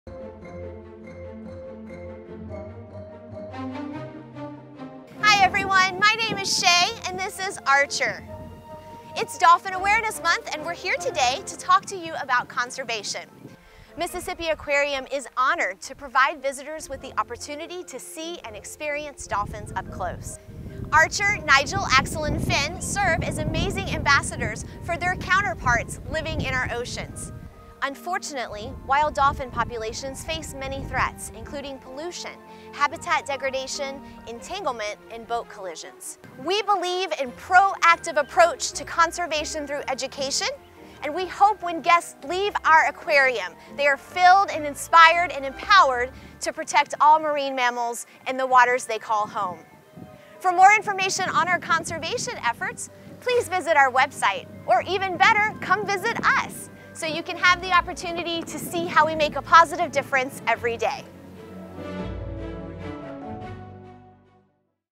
Mississippi Aquarium Dolphin Awareness Month: Conservation
Shea and Archer share how we are protecting dolphins in the wild.